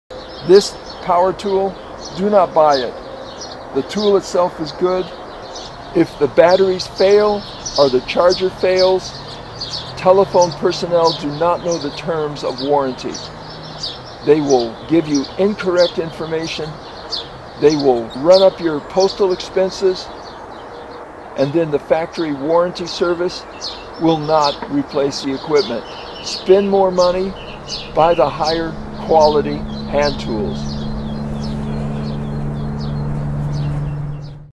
A Failure of DeWalt Service
DeWalt power tools prove their value.  However, if a battery, a charger, or the tool itself fails, the telephone personnel do not know the terms of warranties.

I lost time and dollars packaging and sending a faulty battery for service or exchange.  The service facilty returned the battery without explanation.

After several calls, I found a telephone clerk who told me battery warranties expire after a calendar  year -- no exceptions for long periods of non-use.

Sending the battery for the service center cost $10.  Buying an identical battery cost $22.  The battery I received looked exactly like a DeWalt -- minus the logos and text.

( Since then, the corporation forced their Chinese subcontractors to change the colors of their knock offs. )

Batteries fail.  When the DeWalt batteries fail, the Chinese knocks are excellent.